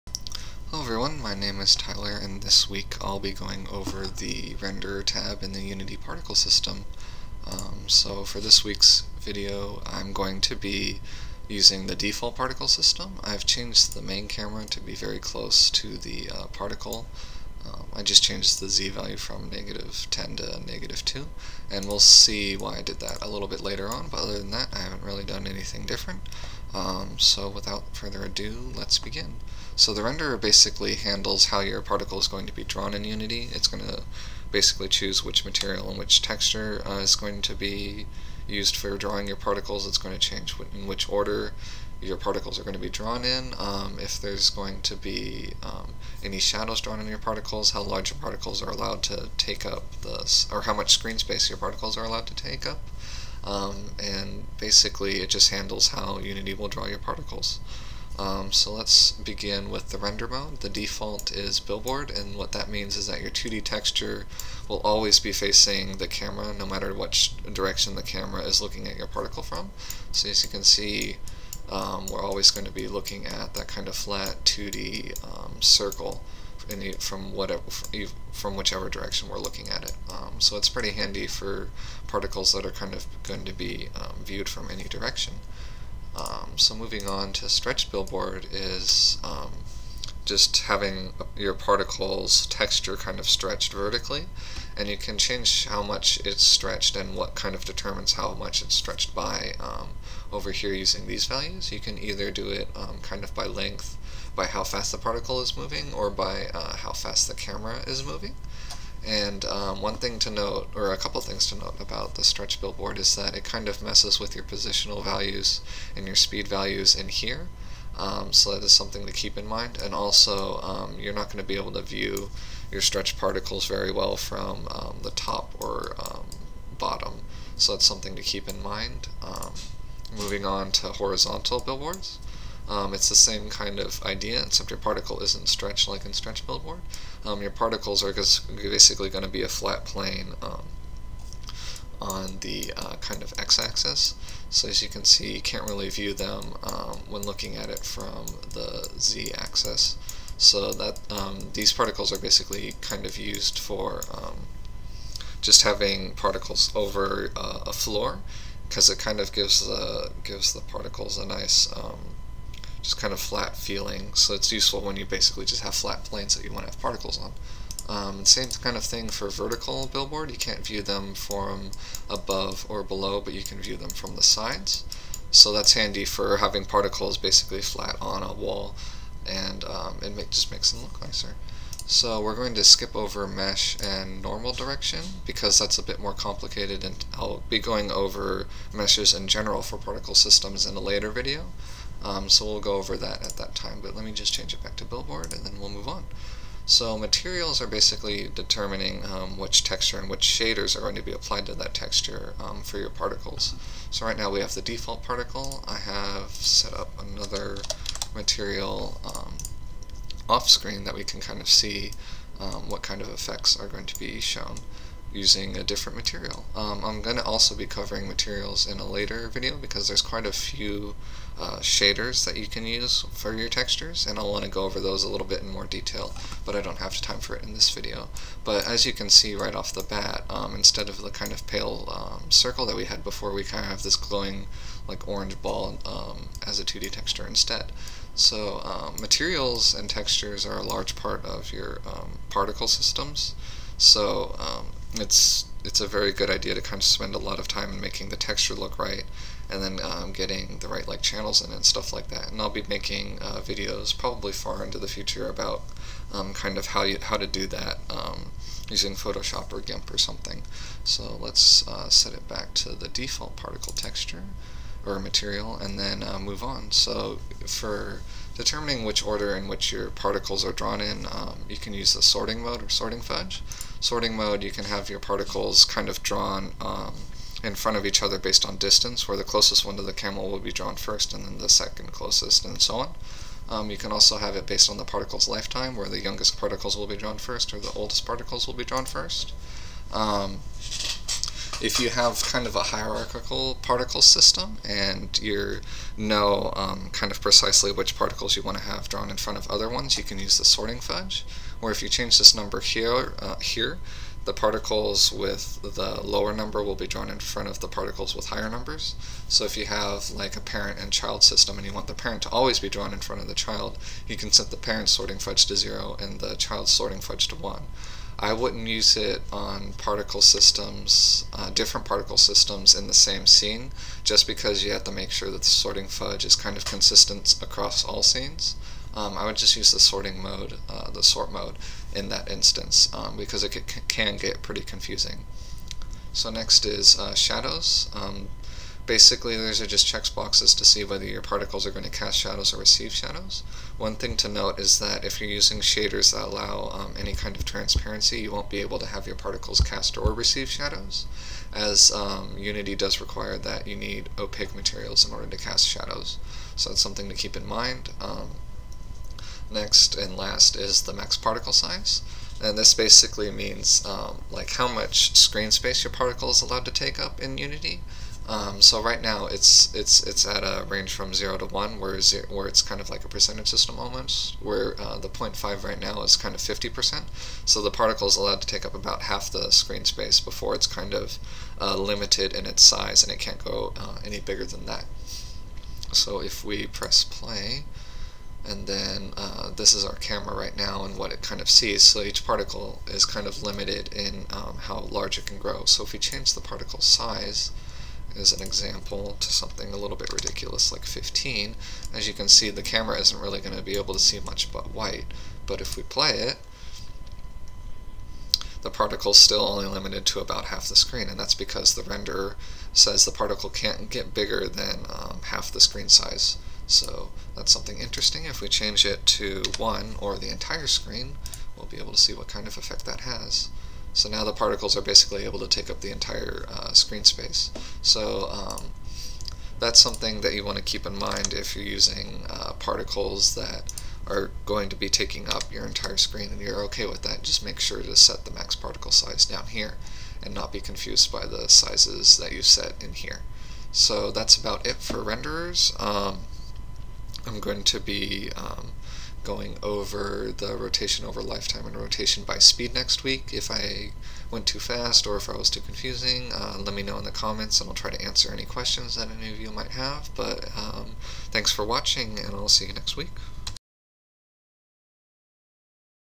Unity's Shuriken Particle System: the Renderer
In this week's video, I explain what the renderer does and how it draws your particle, while my chair squeaks in the background.

Next week, I'll be going over materials. If you have any questions because I went too fast, or if you're just curious about something, feel free to comment below, and I'll answer to the best of my ability!